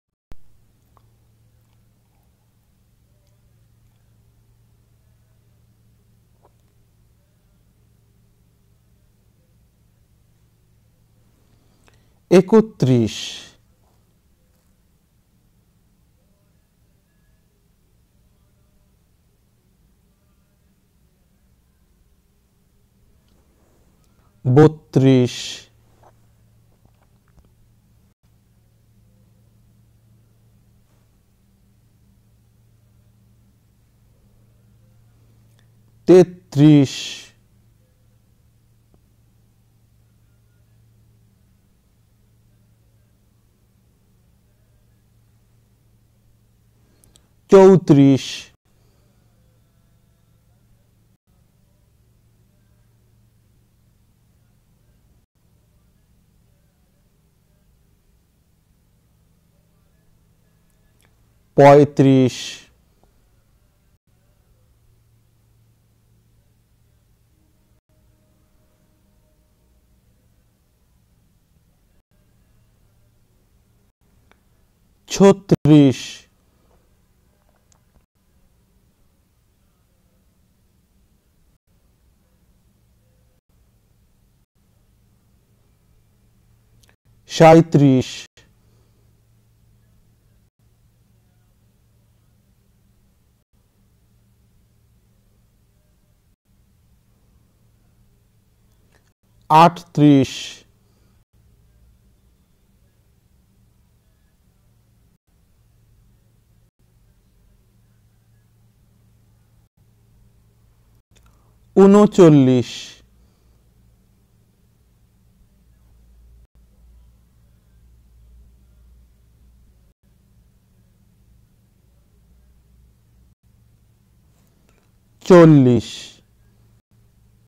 বাংলা সংখ্যা লিখি ৩১-৪০ | How to write Bengali number 31-40
বাংলা ভাষায় প্রাথমিক সংখ্যা লিখন পদ্ধতি ৩১-৪০ যে কোনো মানুষের জীবনে খুবই প্রয়োজনীয়।